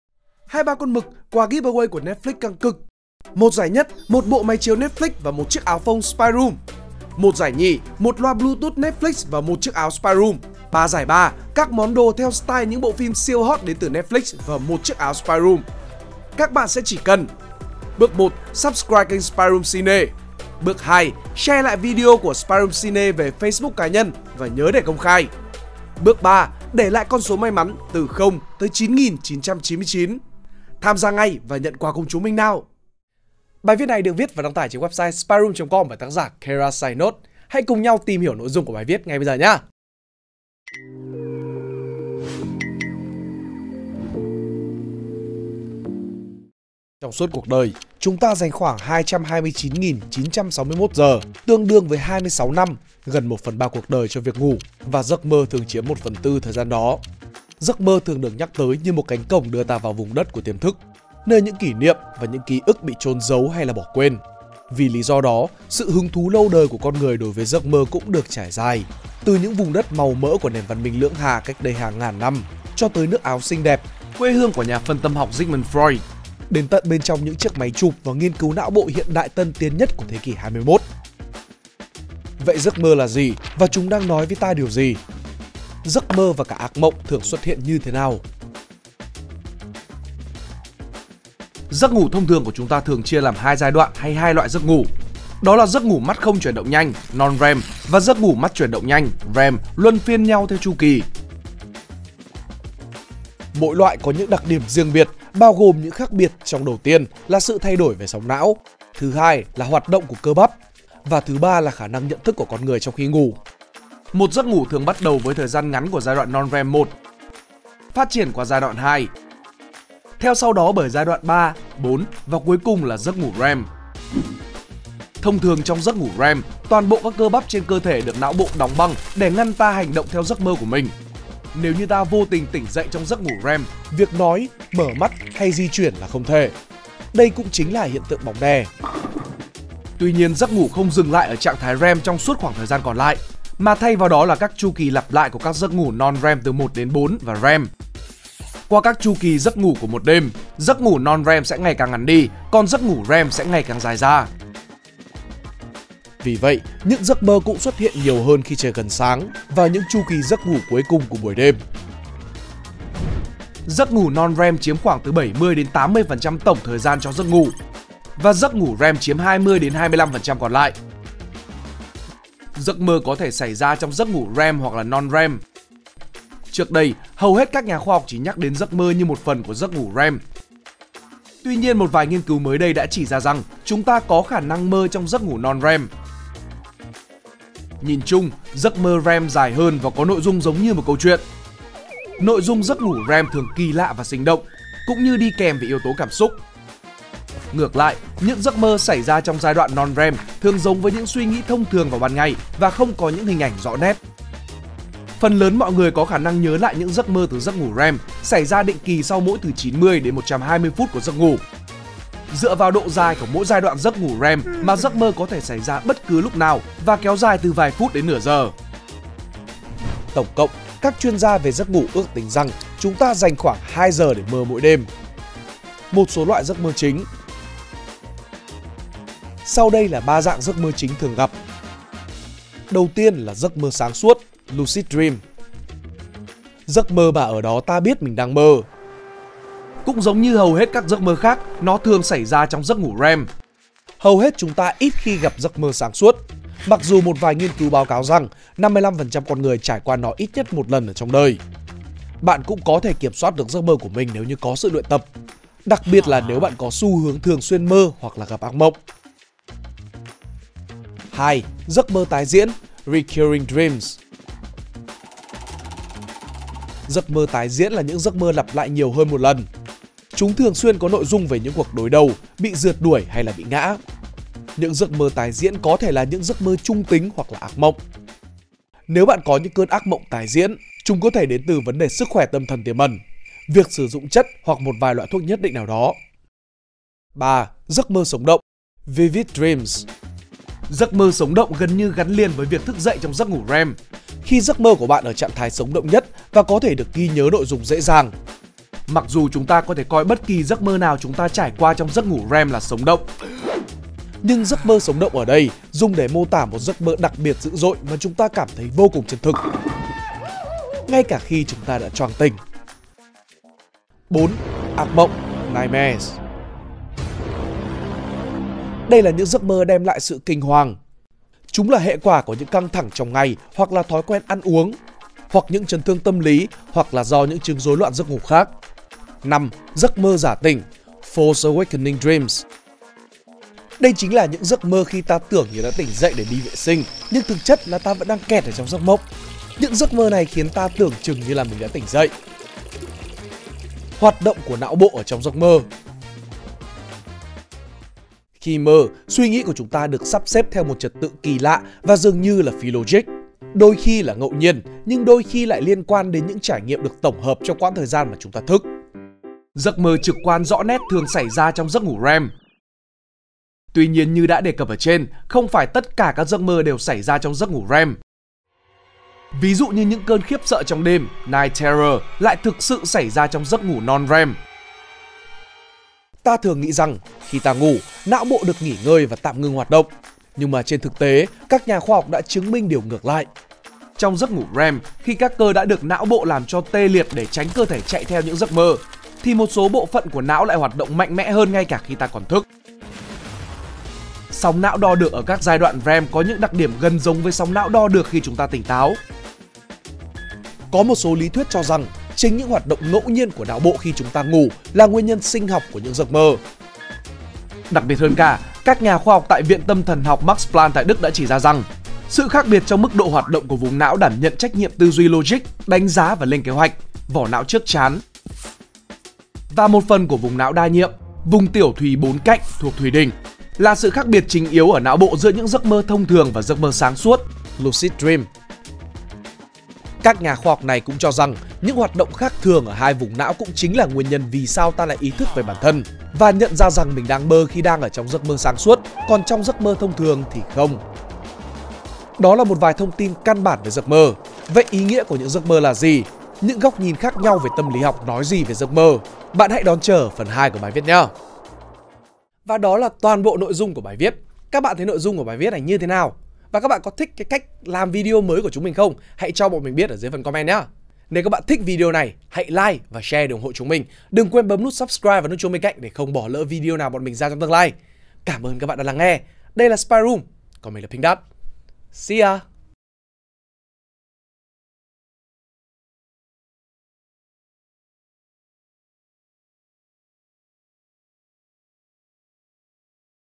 VÌ SAO CHÚNG TA LẠI MƠ? | Keira's PsychNotes | KHOA HỌC
"VÌ SAO CHÚNG TA LẠI MƠ? | Nhện tâm lý | Keira's PsychNotes | SPIDERUM

GIVE AWAY SPIDERUM CINÉ : 2 3 CON MỰC - HỘP QUÀ TỪ NETFLIX CĂNG CỰC

GIẤC MƠ TRONG NHỮNG NGHIÊN CỨU KHOA HỌC | Nhện tâm lý | Keira's PsychNotes | SPIDERUM

Trong suốt cuộc đời, chúng ta dành khoảng 229 961 giờ, tương đương với 26 năm, gần một phần ba cuộc đời cho việc ngủ, và giấc mơ thường chiếm một phần tư thời gian đó. Giấc mơ thường được nhắc tới như một cánh cổng đưa ta vào vùng đất của tiềm thức, nơi những kỷ niệm và ký ức bị chôn giấu hay bỏ quên. Vì lý do đó, sự hứng thú lâu đời của con người đối với giấc mơ cũng được trải dài từ những vùng đất màu mỡ của nền văn minh Lưỡng Hà cách đây hàng ngàn năm, cho tới nước Áo xinh đẹp, quê hương của nhà Phân tâm học Sigmund Freud, đến tận bên trong những chiếc máy chụp và nghiên cứu não bộ hiện đại và tân tiến nhất của thế kỉ XXI. Vậy, giấc mơ là gì và chúng đang nói với ta điều gì?
Cùng tìm hiểu sách ""Seneca: Những Bức Thư Đạo Đức - Chủ Nghĩa Khắc Kỷ trong đời sống"" tại:
Ghé Nhà sách Spiderum trên SHOPEE ngay thôi các bạn ơi:
Bài viết: GIẤC MƠ TRONG NHỮNG NGHIÊN CỨU KHOA HỌC
Được viết bởi: Keira's PsychNotes
Giọng đọc: Pinkdot
Editor: Khang, Hikari, Pinkdot, Nguyễn Sơn